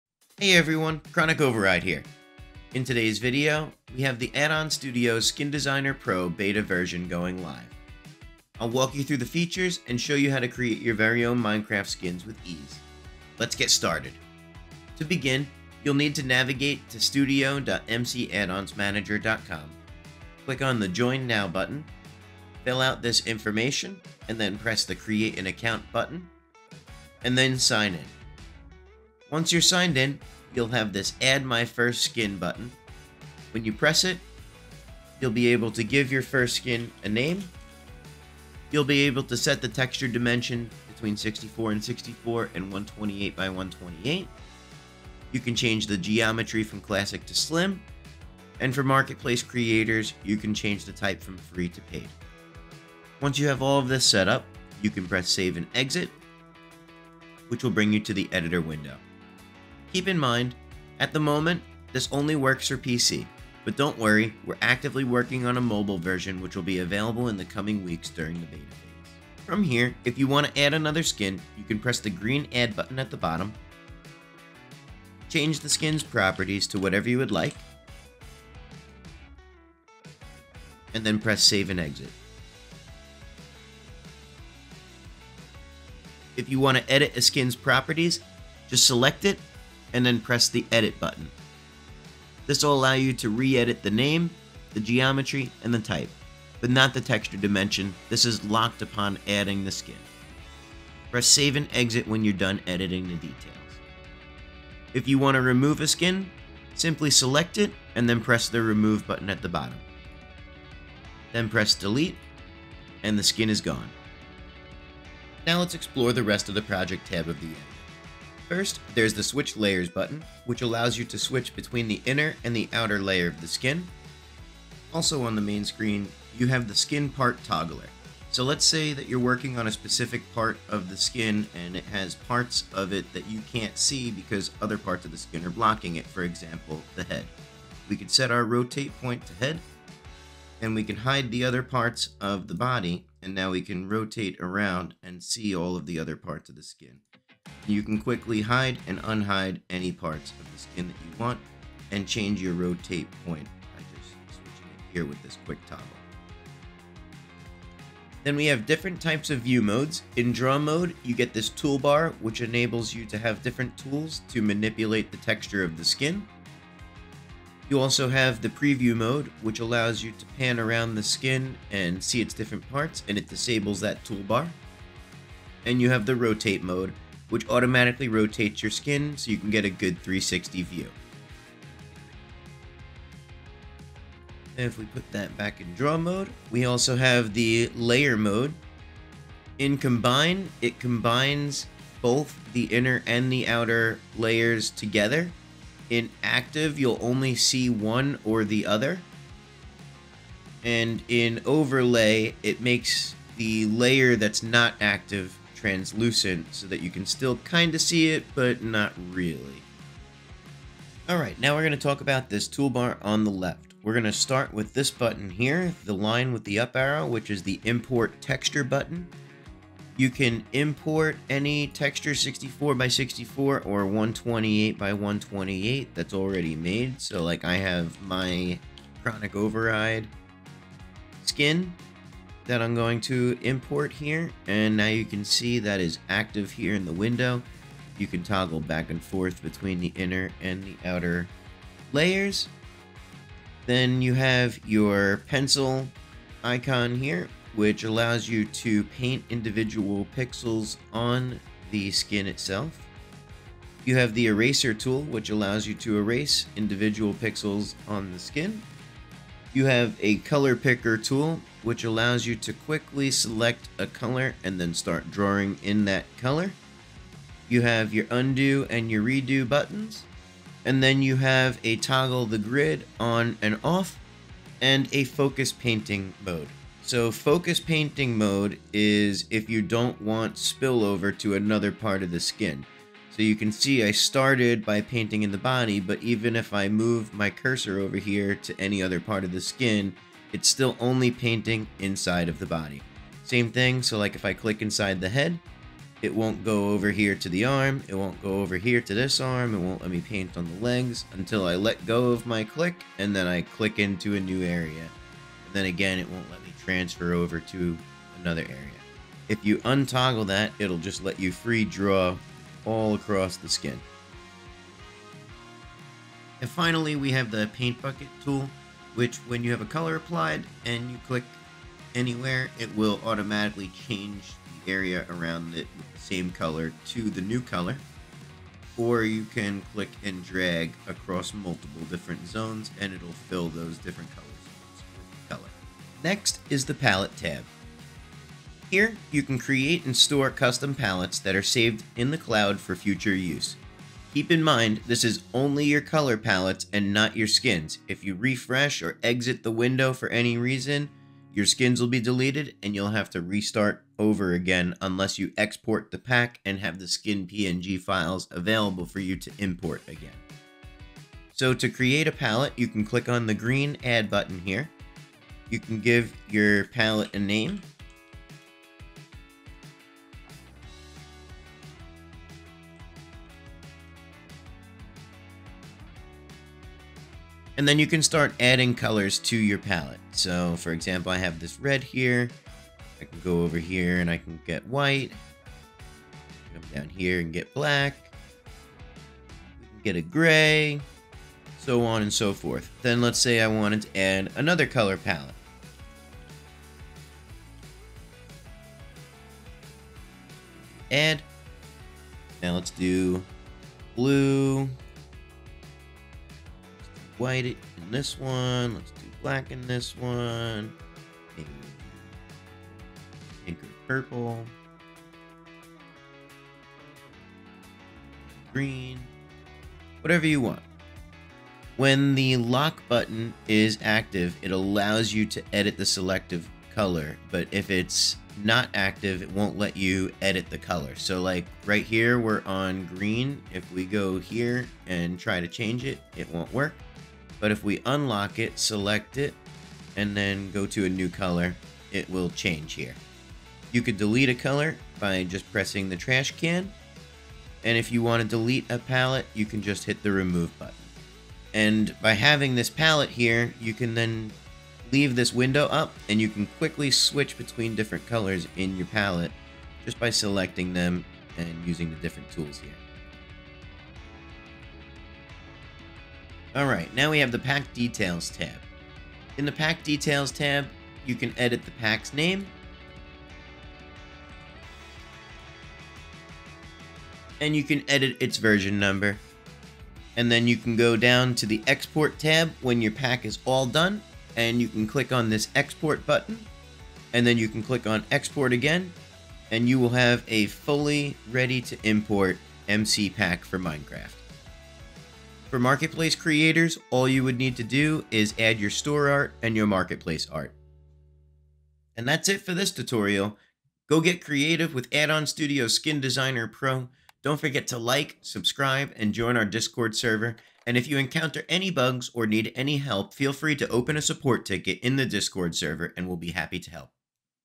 How to Create and your own minecraft Skin Packs with Addons Studio Skin Designer! BETA NOW LIVE!
Welcome to the full tutorial for Addon Studio Skin Designer Pro! 🎨 Whether you're new to Minecraft skin creation or a seasoned designer, this video will walk you through all the essential tools and features of the Skin Designer Pro to help you create your own custom Minecraft skins with ease. 🚀

In this tutorial, you’ll learn:
0:00 | Intro
0:14 | How to set up your account and start creating skins
0:28| Adding your first skin including texture dimensions, geometry, and type
1:09 | Adding Additional Skins, editiing skin details, and removing skins
1:58 | Switching between inner and outer skin layers
2:03 | Skin Part Toggler
2:38 | Exploring the different view modes
3:05 | Navigating different layer modes: Combine, Active, and Overlay
3:30 | Mastering the Toolbar! Pencil, Eraser, Paint Bucket, and more!
4:34 | Focus Painting Mode
5:15 | Paint Bucket Tool
5:36 | Creating and managing custom color palettes 🎨
7:46 | Pack Details Tab
8:00 | Exporting your skin packs for Minecraft and Marketplace creators 🛒

This step-by-step guide covers everything you need to know to make amazing skins. The beta is live now, so start creating today!

We can’t wait to see what skins you create!